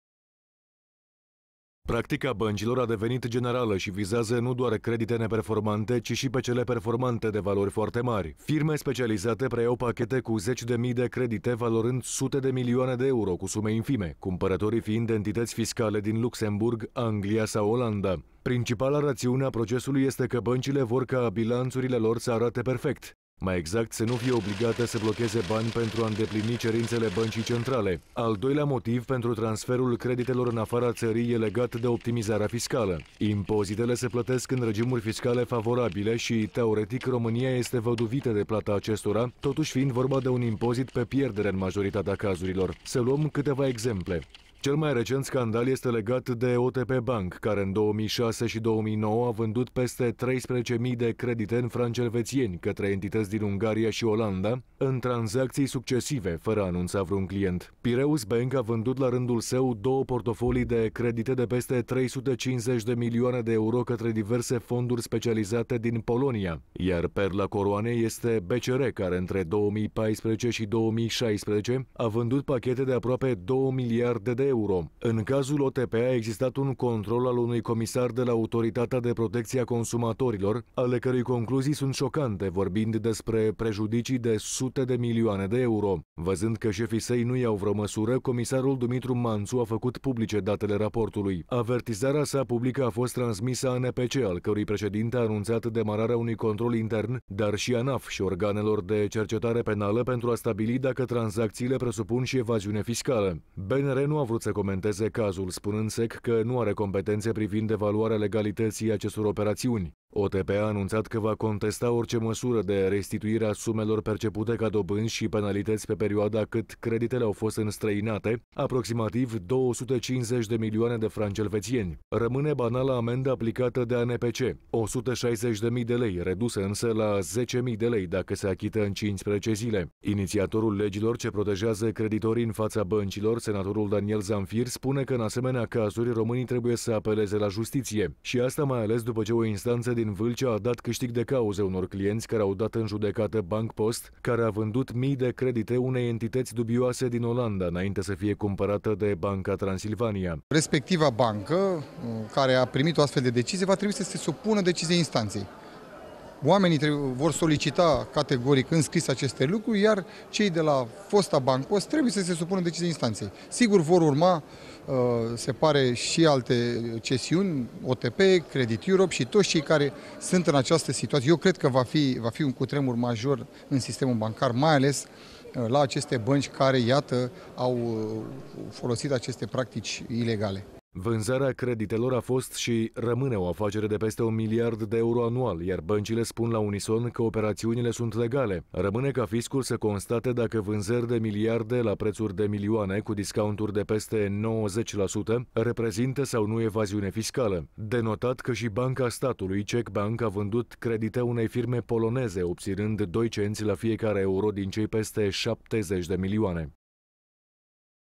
cesionare de credite
Practica băncilor de a-și cesiona portofoliul de credite a devenit generală și vizează nu doar credite neperformante, ci și pe cele performante, de valori foarte mari.